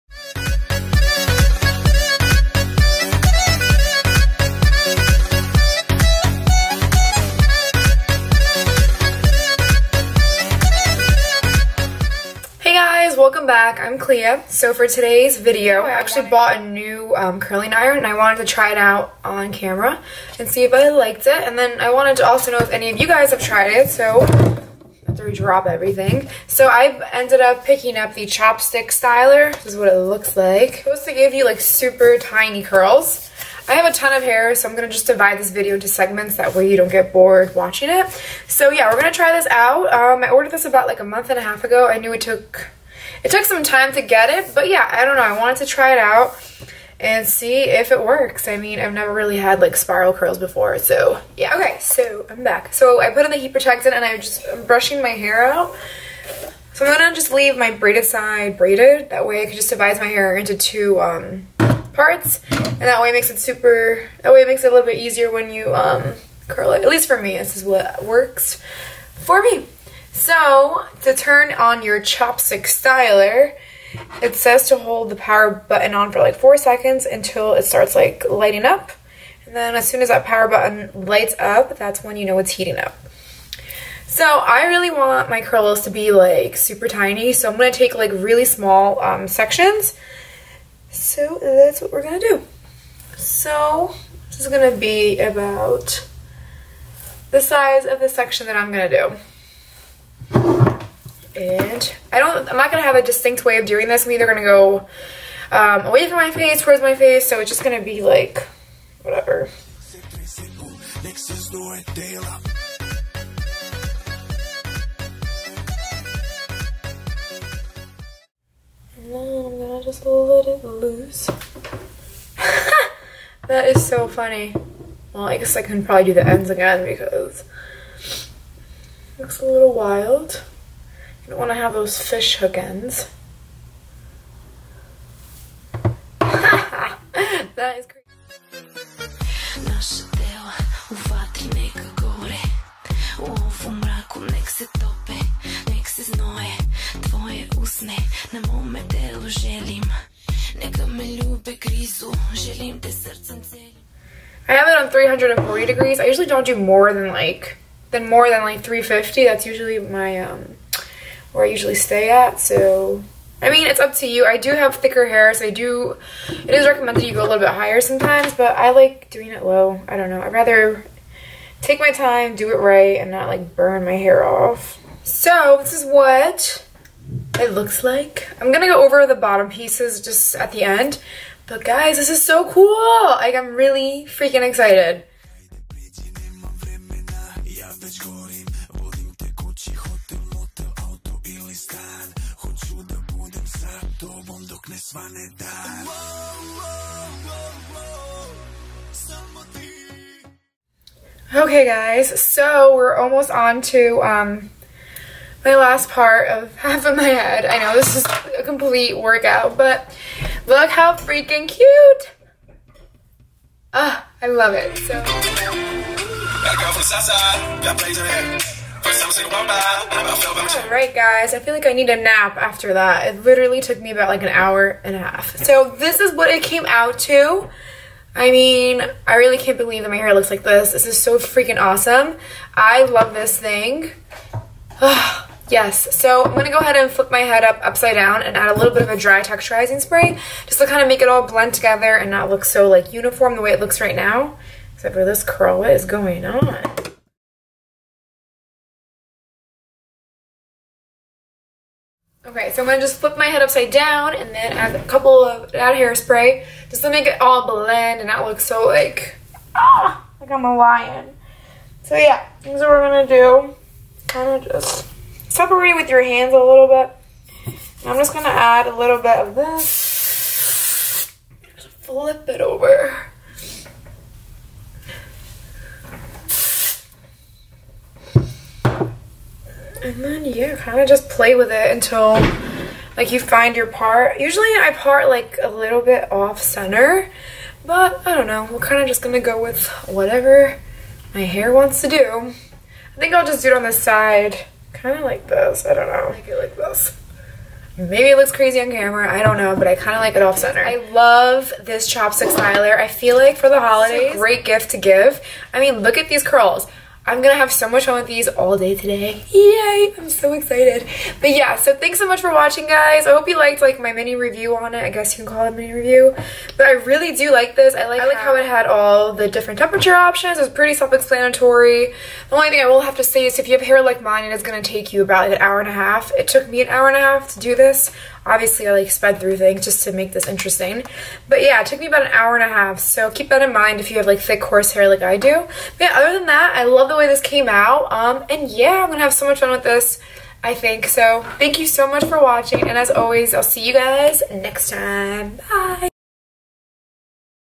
Chopstick Styler Curling Wand (Review)
Hey ladies! Sooo I tried this wand and I loved it, cant even tell you how many compliments I received on these curls! Yay!

"it may have been just a moment to you, but it changed every single one that followed for me"